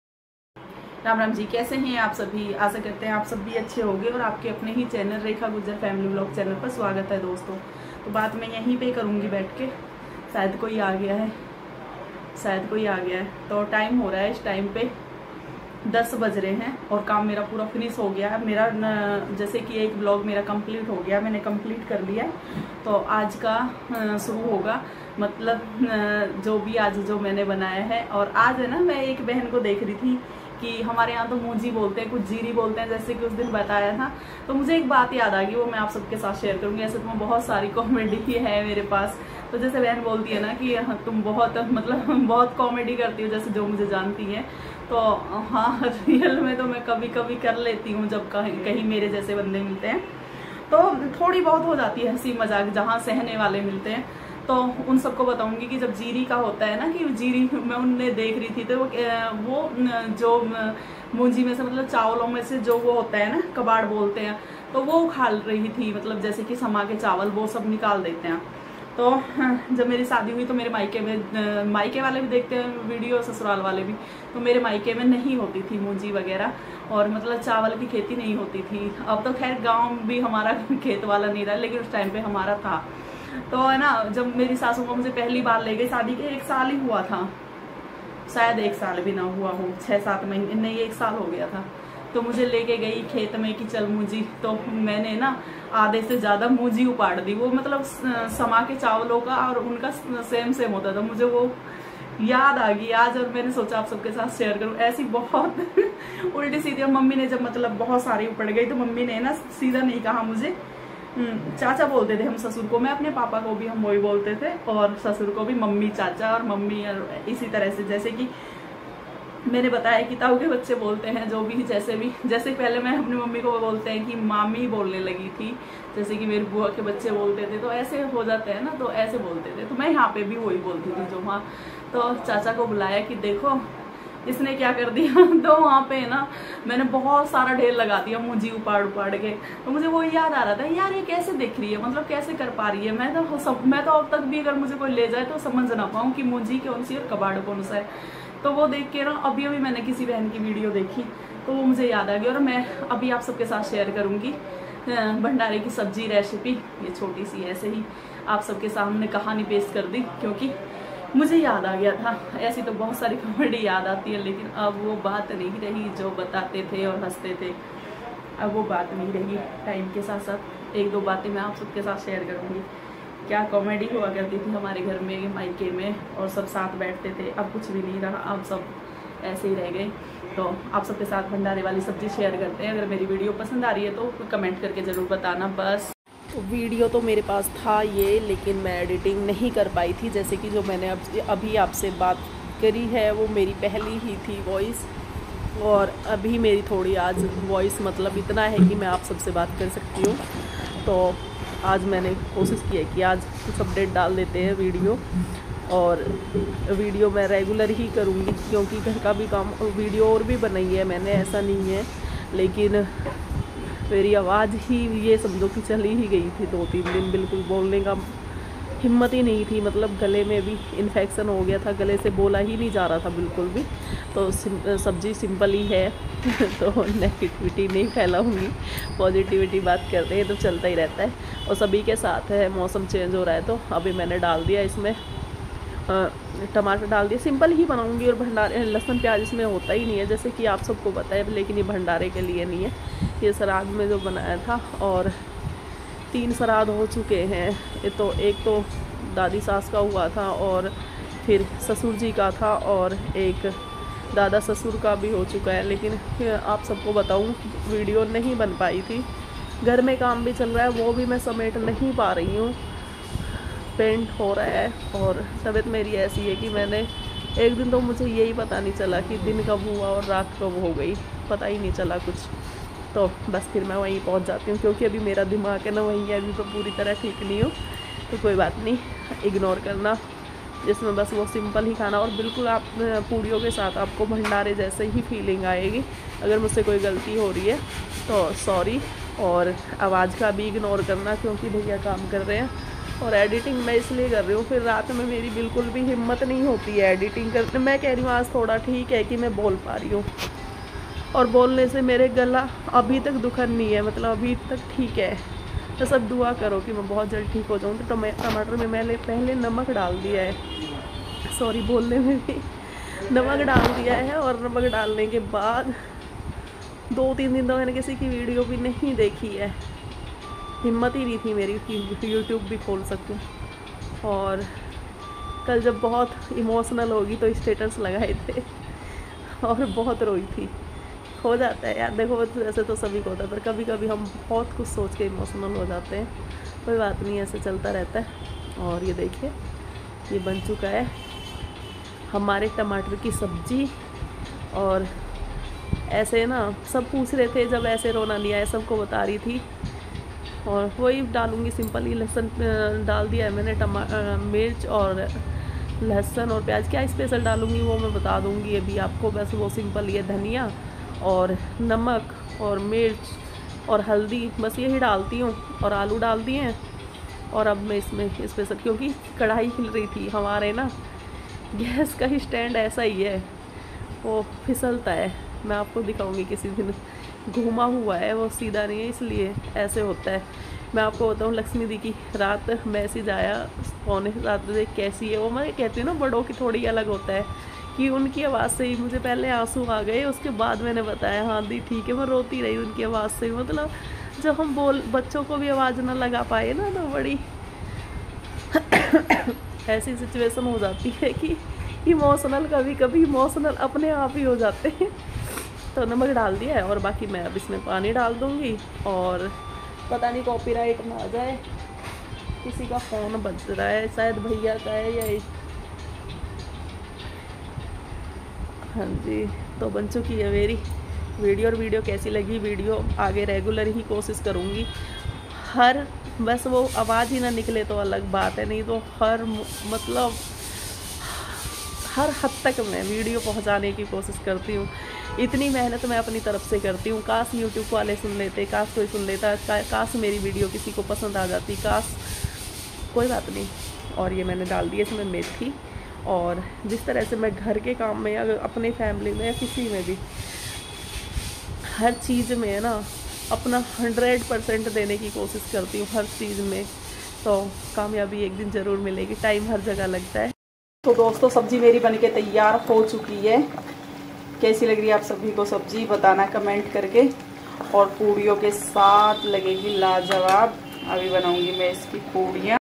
सच मानो तो 3 दिन के लिए आवाज ही चली गई थी कुछ पहले रिकॉर्ड था थोड़ी सी एडिटिंग पसीने छूट गए
rekha gurjar new vlog